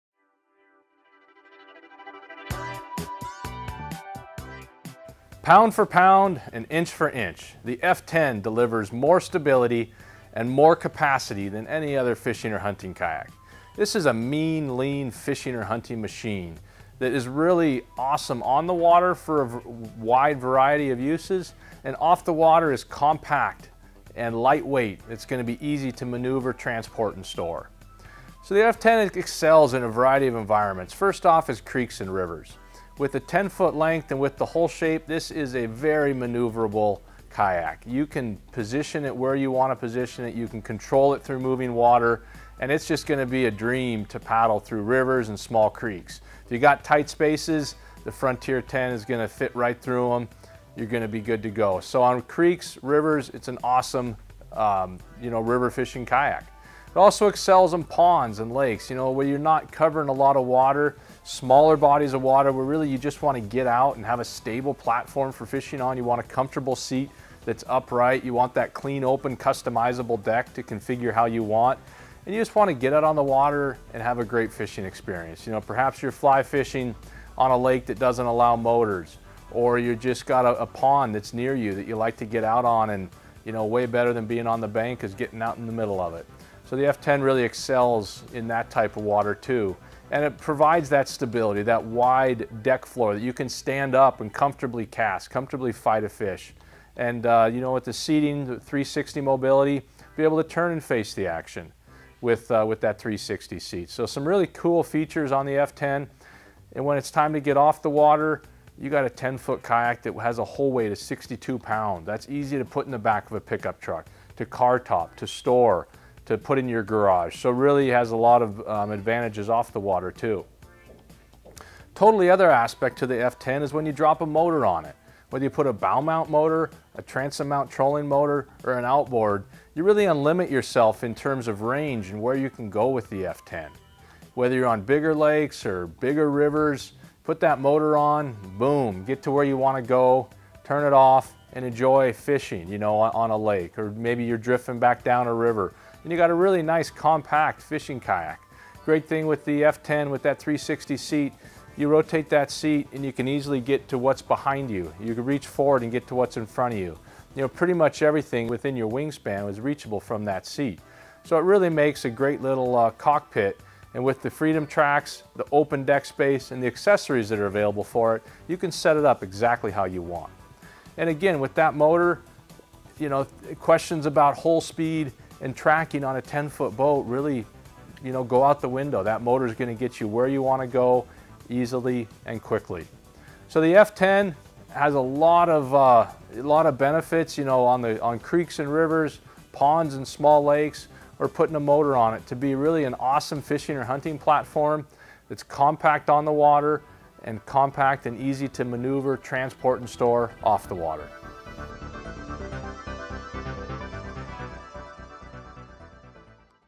Meet the Model - The F10 - Fishing & Hunting Kayak
The NuCanoe F10 is a compact fishing kayak with a spacious & customizable deck.  It is lightweight off the water and nimble on the water, perfect for ponds, lakes, rivers, and pickup trucks!  Add a motor to the F10 and you've got small fishing boat that will go anywhere you want to go!